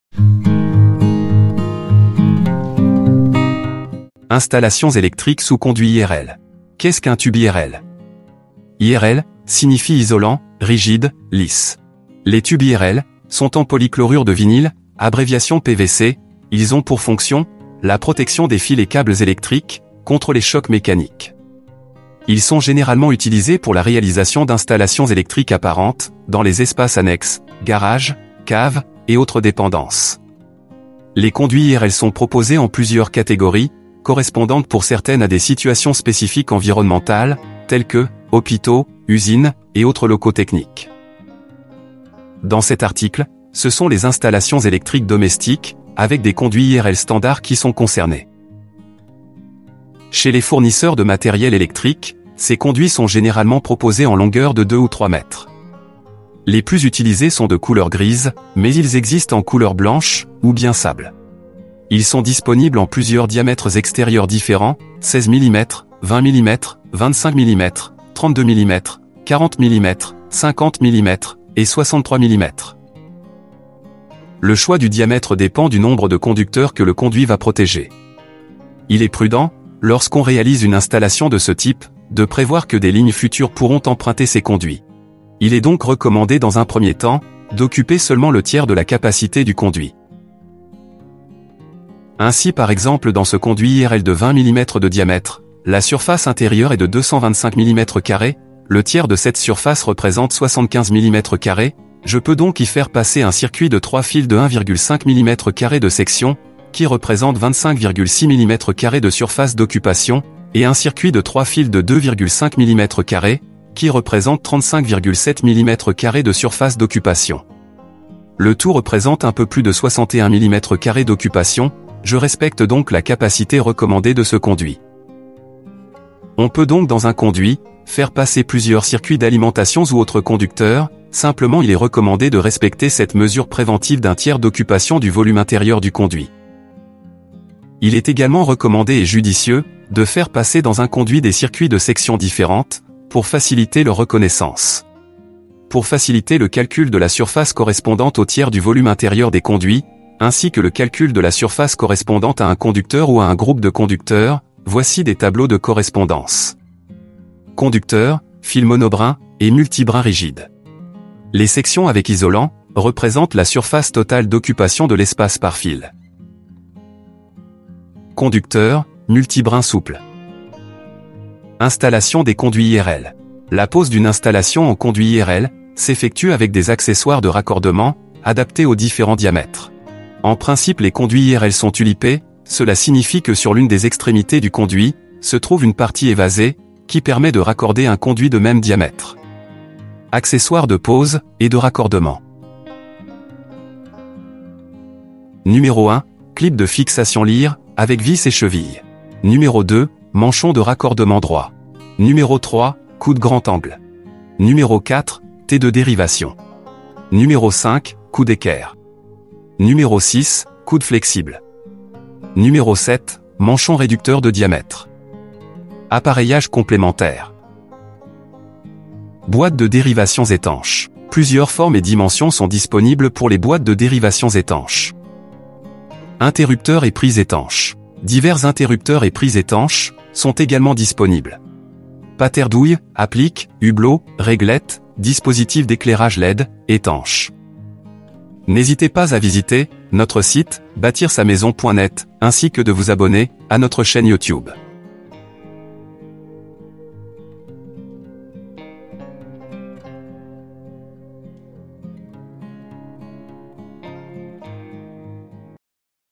Installations électriques sous conduits I R L
Vidéo : Installations électriques sous conduits I R L
Les tubes IRL sont en Polychlorure de Vinyle, abréviation  PVC, ils ont pour fonction, la protection des fils et câbles électriques contre les chocs mécaniques.
Ils sont généralement utilisés pour la réalisation d’installations électriques apparentes, dans les espaces annexes, garages, caves et autres dépendances.
Les conduits IRL sont proposés en plusieurs catégories correspondantes pour certaines à des situations spécifiques environnementales telles que : hôpitaux, usines, et autres locaux techniques.
Pour consulter la version écrite et illustrée rendez vous sur le blog :
 « j’aime »  Merci !